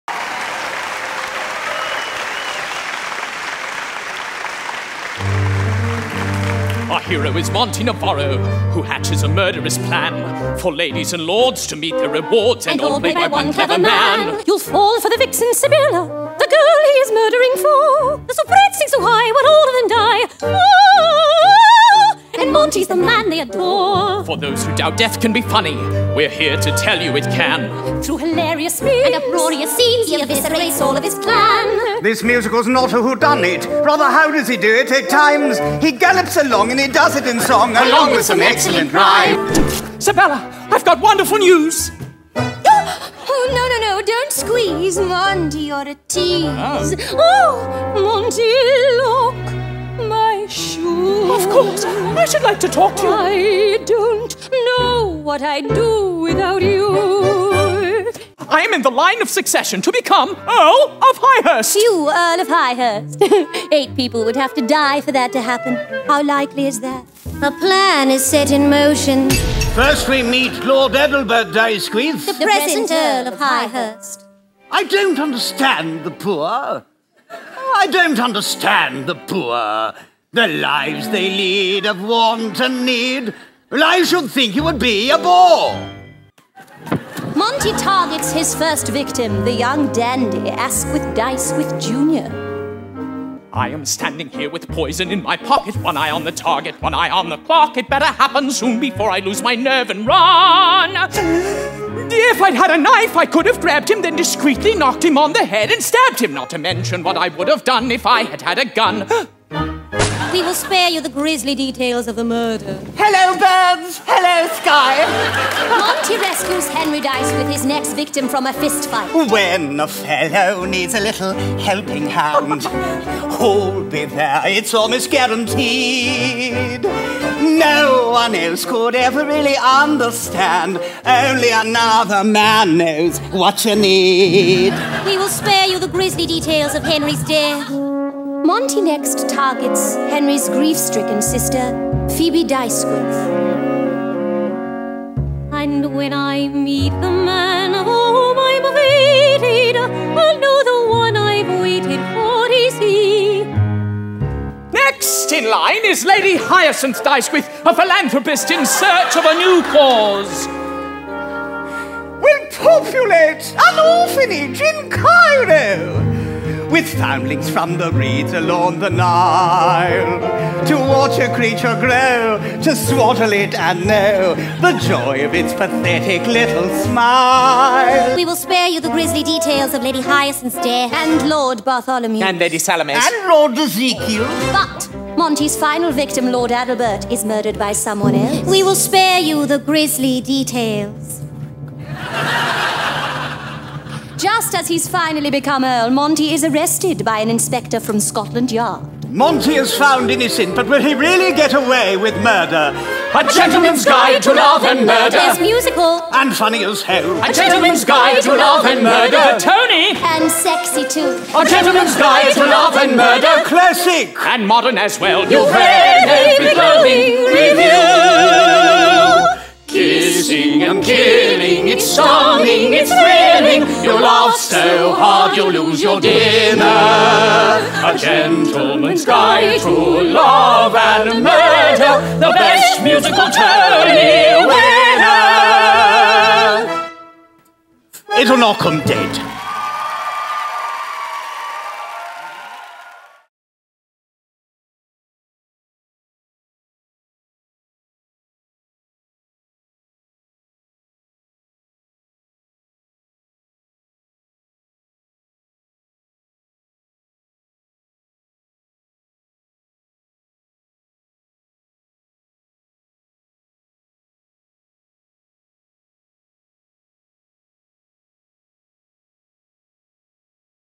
A Gentleman's Guide to Love & Murder in 4 Minutes
Want to know what A Gentleman's Guide to Love & Murder is about? Only have 4 minutes? then this video is for you.